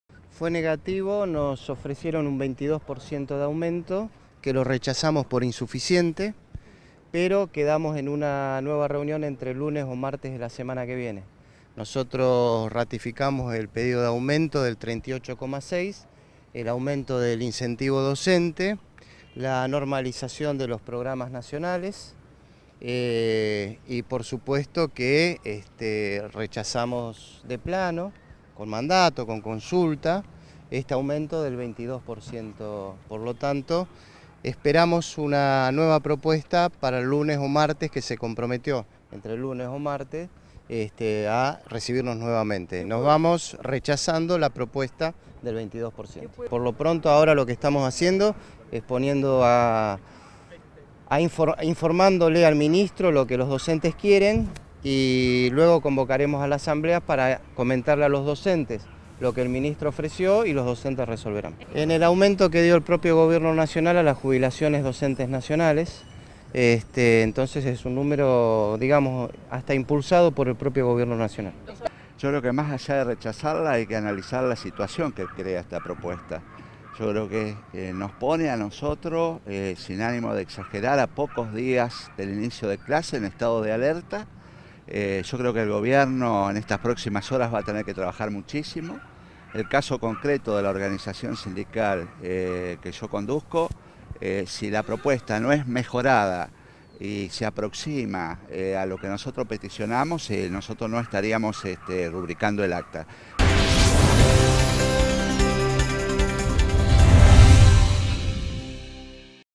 Gremios docentes rechazaron oferta salarial del 22% de aumento y continúan las negociaciones
Los cinco sindicatos docentes con representación nacional rechazaron hoy la oferta de incremento salarial del 22 por ciento en el salario del maestro de jornada simple sin antigüedad en el marco de las paritarias, por lo que con las autoridades del Ministerio de Educación acordaron una nueva reunión entre el lunes y martes próximo, informaron fuentes sindicales.

Eduardo López, miembro paritario por Ctera, dijo a Télam que "la oferta del 22 por ciento fue considerada insuficiente y rechazada por los gremios, que habíamos planteado un 38,6 por ciento de incremento, por lo que el gobierno nacional presentará una nueva oferta entre el lunes y martes próximo".

"Ratificamos el pedido de incremento del incentivo docente y la normalización de programas nacionales, además del aumento del 38,6, que se basa en el que dio el Gobierno nacional a los jubilados", apuntó.López subrayó que "hay muchas jurisdicciones que no están convocando a paritaria porque están esperando los resultados de la paritaria nacional, por lo que tenemos doble responsabilidad".

Sergio Romero, titular de la Unión de Docentes Argentinos (UDA), dijo a Télam que esperaban "un esfuerzo mayor de parte del Gobierno nacional".El sindicalista consideró que el Gobierno nacional "va a tener que trabajar muchísimo para mejorar la oferta" y adelantó que si la misma "no mejora o no se aproxima a lo que pedimos, la UDA no firmará el acta".

"Estamos convencidos de que el diálogo debe prevalecer, pero representamos a cientos de familias y vamos a trabajar para que las condiciones mejoren", afirmó.Por su parte Fabián Feldman, de la Confederación de Educadores Argentinos (CEA), apuntó a Télam que "siempre reunirnos es positivo, veníamos con muchas expectativas pero la propuesta resultó insuficiente".

"Hay jurisdicciones que están abonando un salario básico inferior a los 2.200 pesos, por lo que hay muchas cuestiones que resolver todavía", subrayó.Jorge Kalinger, del Sindicato Argentino de Docentes Privados (Sadop), coincidió y agregó que "(Alberto) Sileoni nos dijo que la propuesta del 22 por ciento tiene que ver con la desaceleración de la inflación que se espera para este año".

"De todos modos resulta un número insuficiente y menor al del año pasado, por lo que esperamos que revalúen la propuesta y vendremos a escucharlos la semana que viene", apuntó.

La reunión, que se realizó en el Palacio Sarmiento y fue el segundo encuentro por paritarias, fue encabezada por el ministro de Educación, Alberto Sileoni, junto a otros funcionarios de su gabinete,El primer encuentro se había llevado a cabo el jueves pasado, donde los gremios elevaron la propuesta conjunta para incrementar el sueldo del maestro que recién inicia su actividad en un 38,6 por ciento, lo que llevaría el piso mínimo en todo el país a 6.100 pesos.